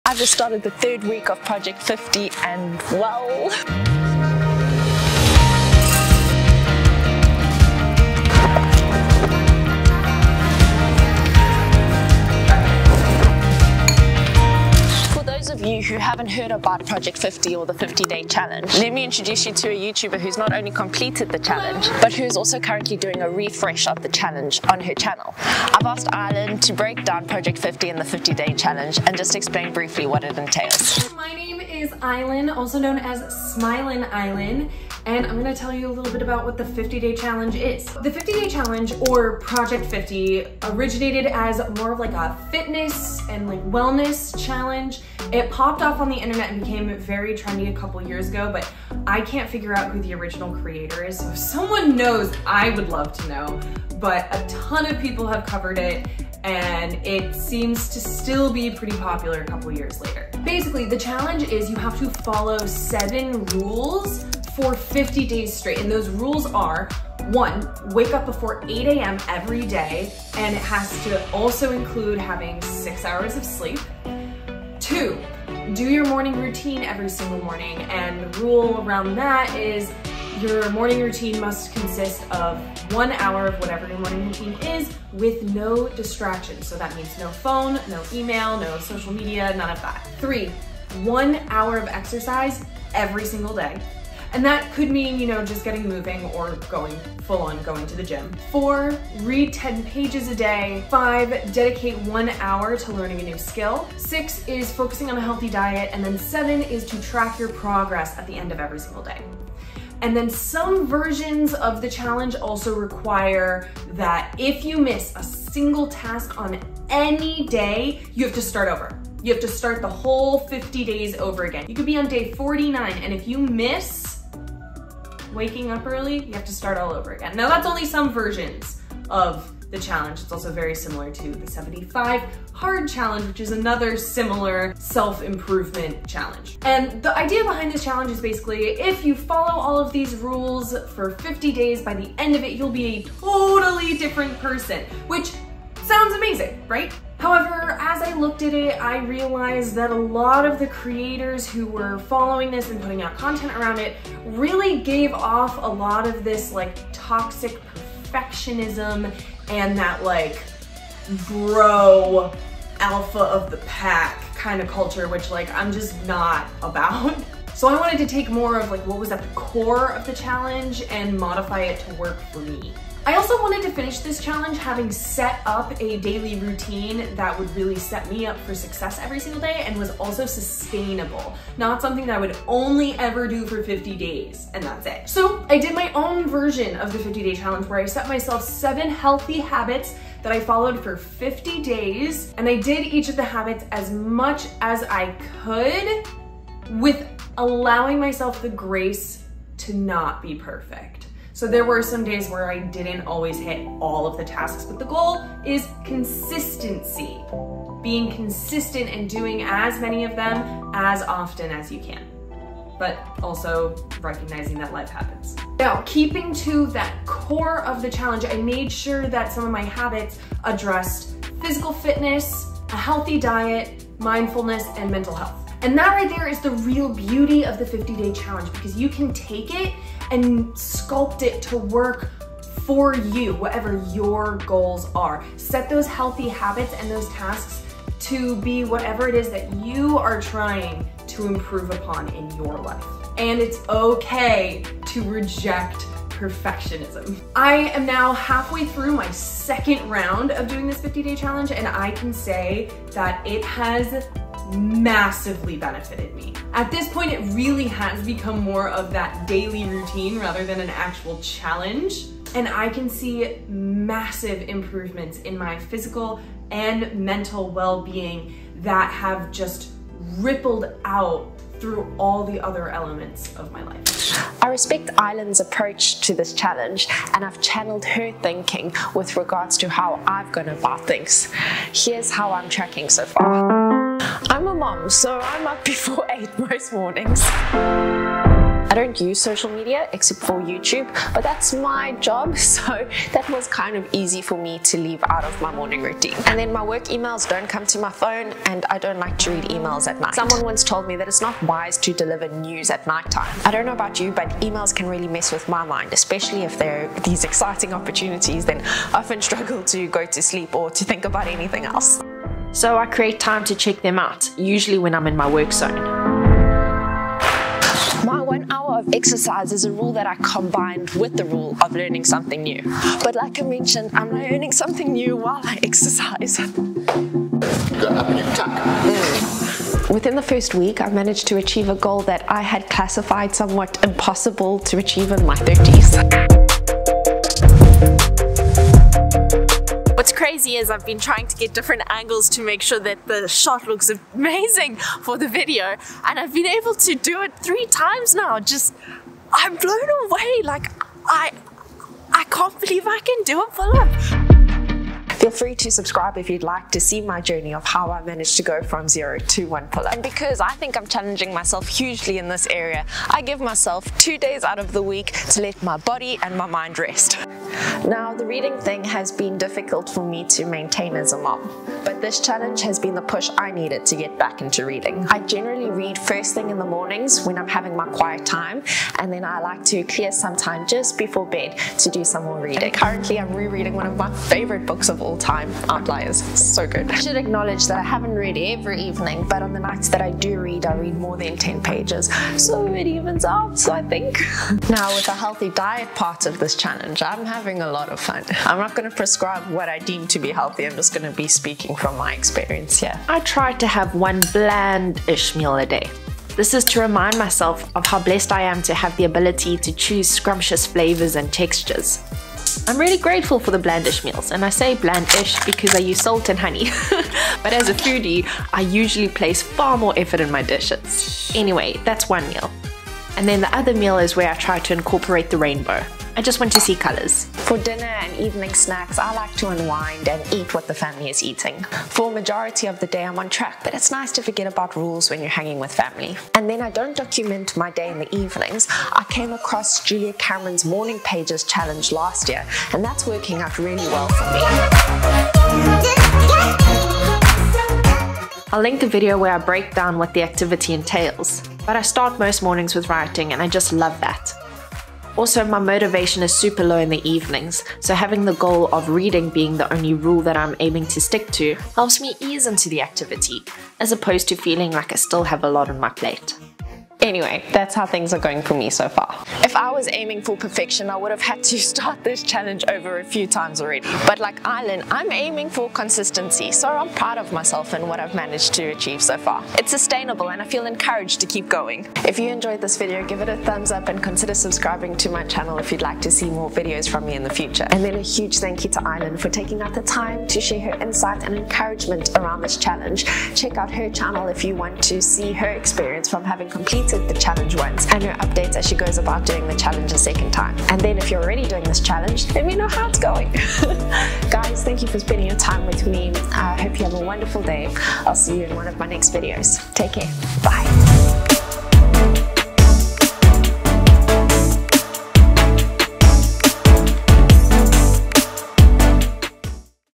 How Project 50 Can Radically Change Your Life
Project 50 Challenge: Transform Your Life in 50 Days! 🌟🚀

Thanks to @SmilinAislinn for sharing her experience with us in the video today.

Ready to embark on a journey that could change your life? Join me in today's video as we dive into the captivating concept of Project 50 – a 50-day challenge that has been hailed as a game-changer for those seeking personal growth, resilience, and a renewed sense of purpose.

🌈 Discover the essence of Project 50 and why this transformative challenge has captured the imaginations of countless individuals around the world. I'll unpack the philosophy behind the 50-day duration, exploring how it aligns with the science of habit formation and the potential for profound positive change in a relatively short span.

🚀 Learn about the diverse range of activities and goals that participants set for themselves during Project 50, from health and fitness milestones to creative pursuits and mindset shifts. The beauty of this challenge lies in its adaptability to various aspects of life, allowing each participant to tailor their experience to their unique aspirations.

💪 Uncover the stories of individuals who have embraced Project 50 and emerged with newfound resilience, confidence, and a fresh perspective on life. I'll share insights into the positive impact this challenge has had on mental health, motivation, and the cultivation of habits that endure beyond the 50-day mark.

Share your thoughts or experiences with challenges like these in the comments below – have you ever embarked on a transformative journey, and how did it shape your life? 💬🌟